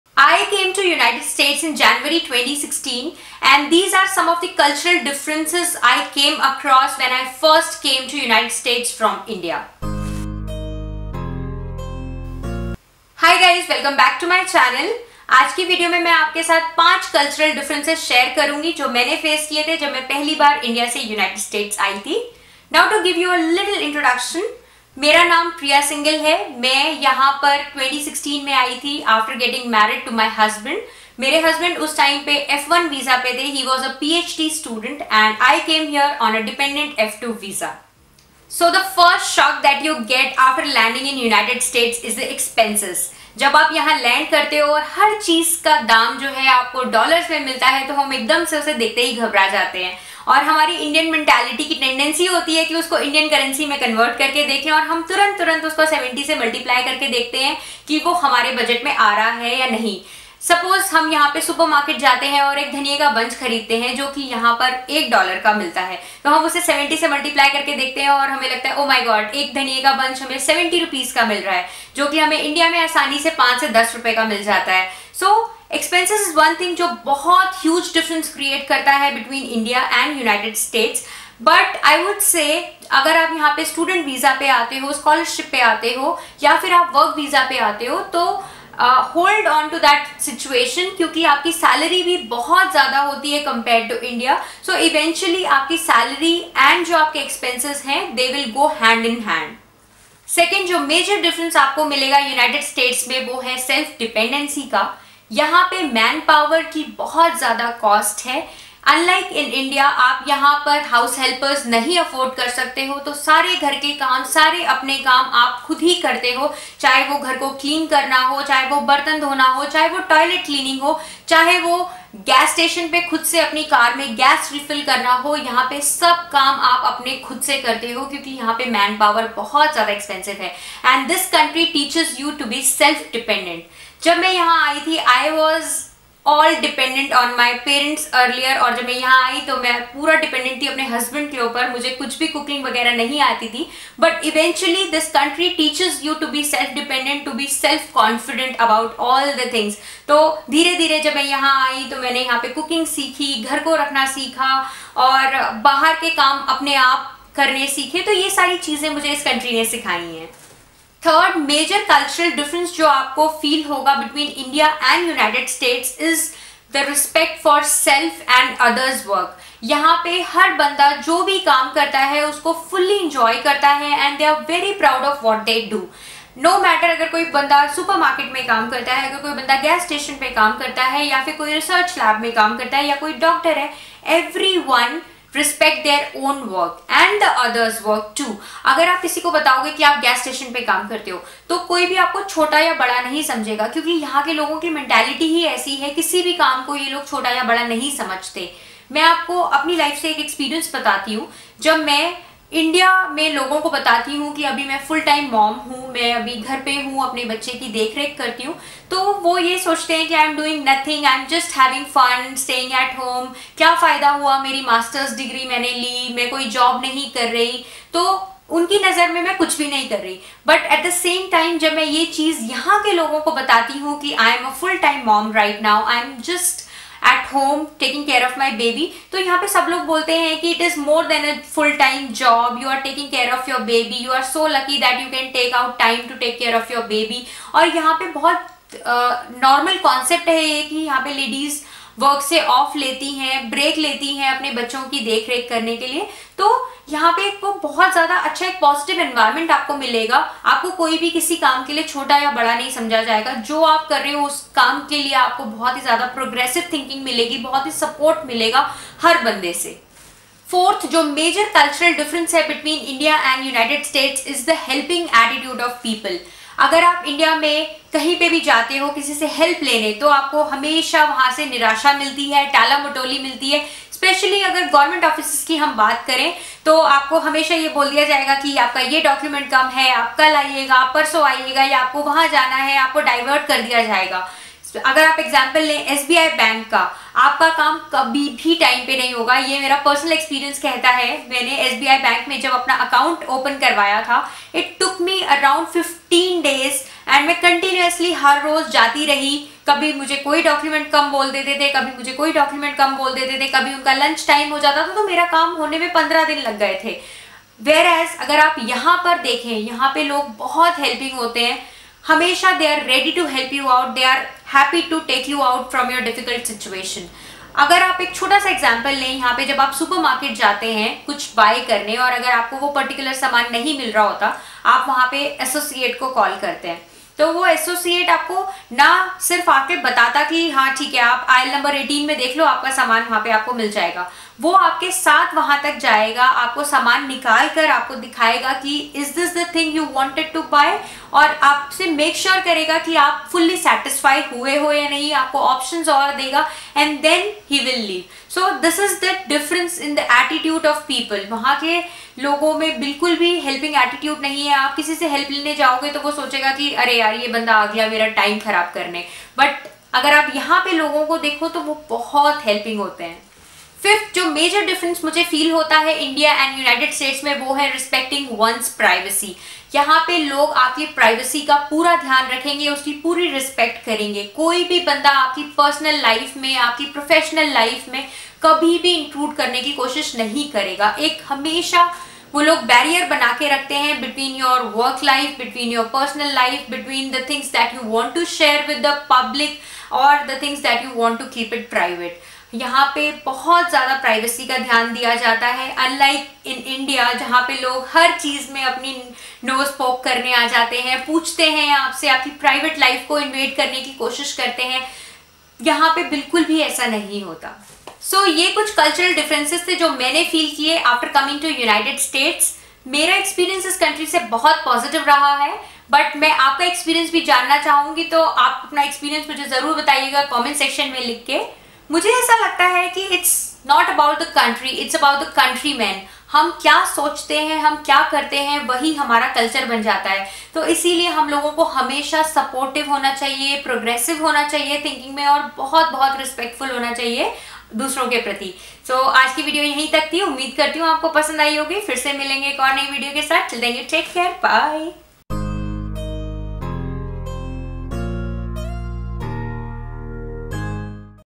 5 Culture Shocks Entering USA from India | Indians In USA
It's been over 3 years since I came to the US after getting married in 2015. I entered the US first in 2016 on a dependent F2 visa. There have been many culture shocks that I observed since then, and I thought it would be useful to talk about some of them. :)

Title: 5 Culture Shocks Entering USA from India | Indians In USA